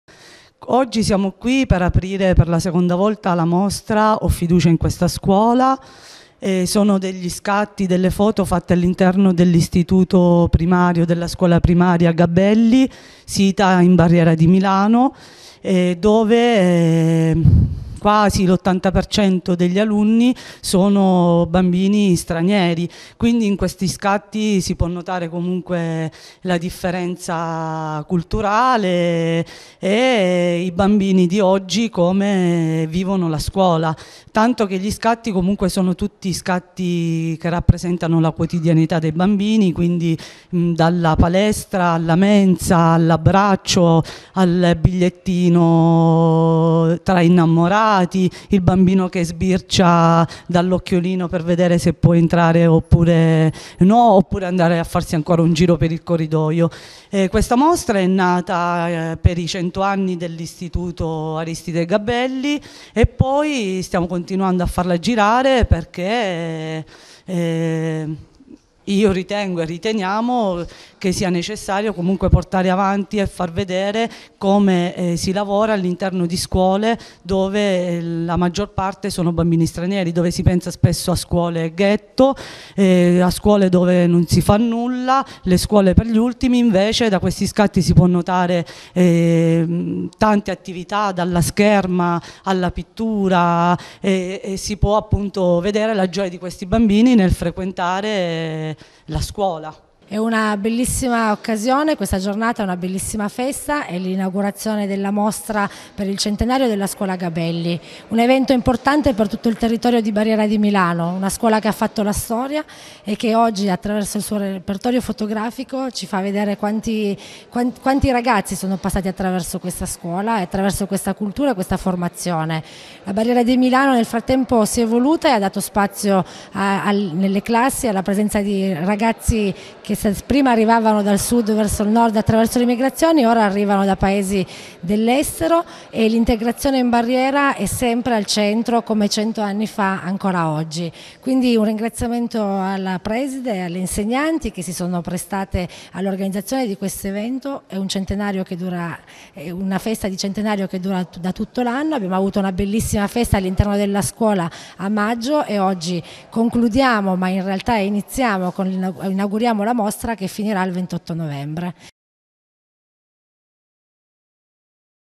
Torino: La scuola Gabelli in mostra - GRP Televisione
Ci sono scuole definita di confine che, grazie all'impegno dei loro insegnanti,   portano avanti un'intensa e utile attività didattica per i loro alunni. Tra queste c'è anche l'istituto scolastico "aristide Gabelli". In occasione del centenario di fondazione della scuola, è stata aperta al pubblco una mostra fotografica che ne testimonia il passato e il presente.La mostra resterà aperta fino al 28 novembre.